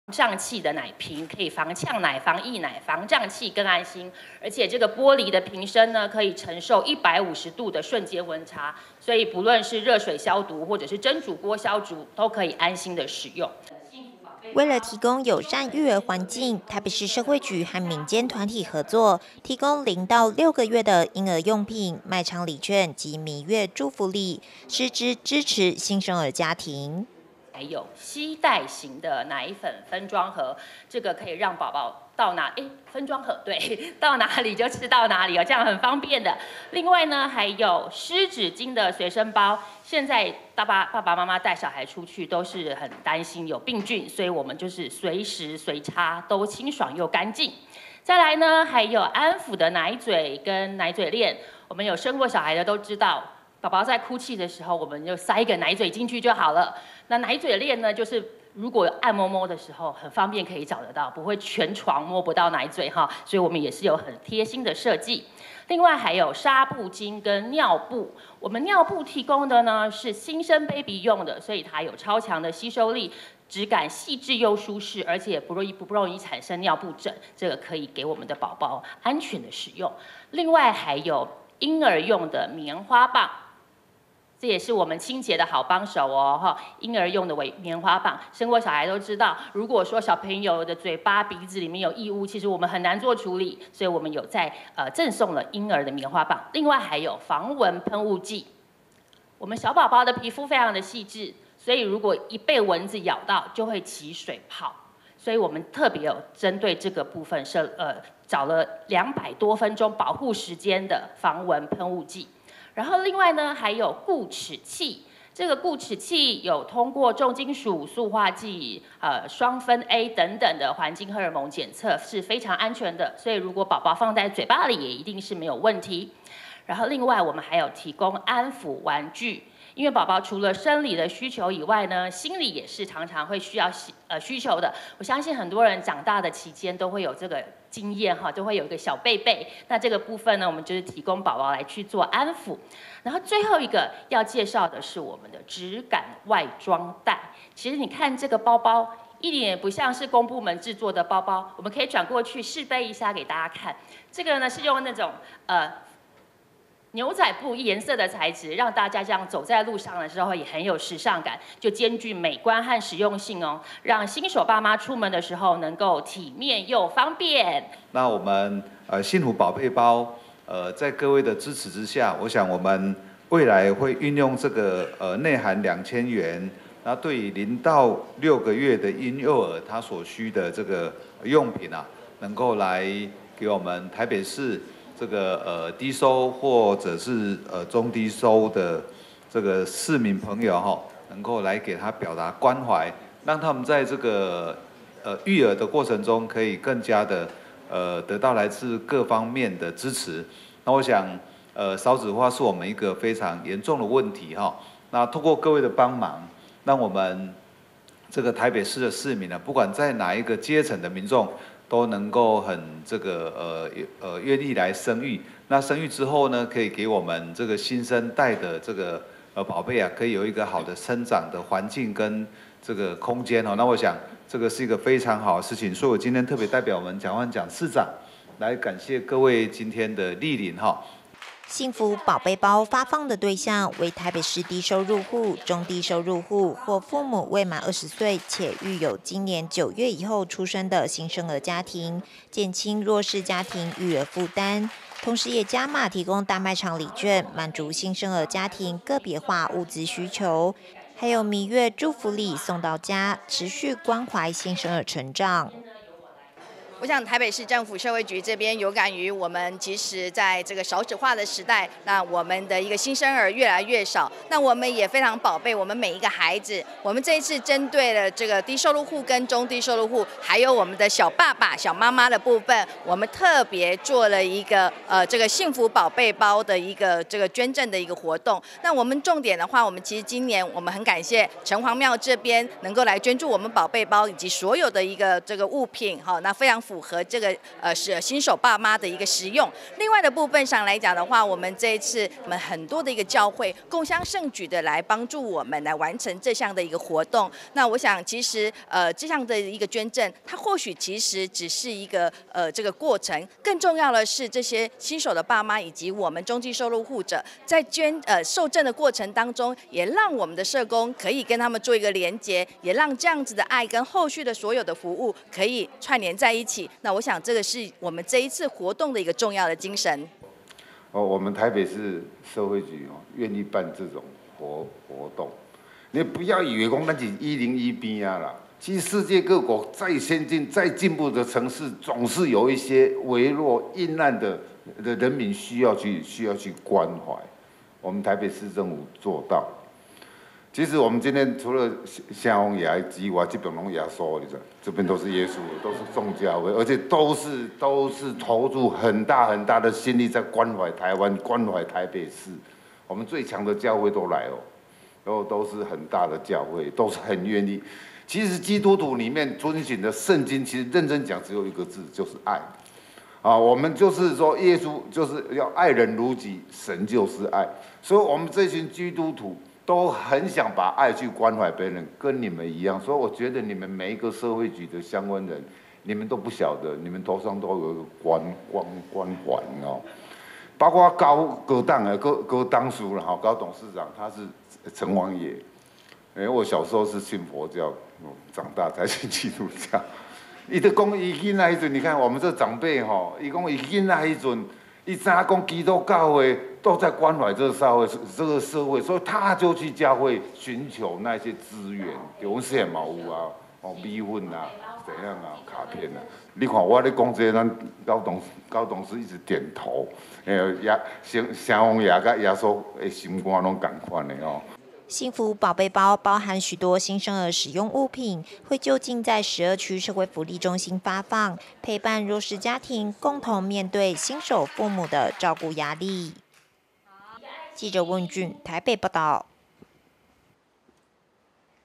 蔣萬兒|育兒政策|免費拿|台北寶貝作伙疼惜 新生兒實用物資免費領
台北寶貝作伙疼惜 新生兒實用物資免費領

為提供友善育兒環境，台北市社會局和民間團體合作，提供0到6個月的嬰兒用品、賣場禮券及彌月祝福禮，實質支持新生兒家庭。

幸福寶貝包發放的對象為台北市低收入戶、中低收入戶或父母未滿20歲且育有今年9月以後出生的新生兒家庭，減輕弱勢家庭育兒負擔，同時也加碼提供大賣場禮券，滿足新生兒家庭個別化物資需求，還有彌月祝福禮送到家，持續關懷新生兒成長。

幸福寶貝包包含許多新生兒實用物品，會就近在12區社會福利中心發送，陪伴弱勢家庭共同面對新手父母的照顧壓力。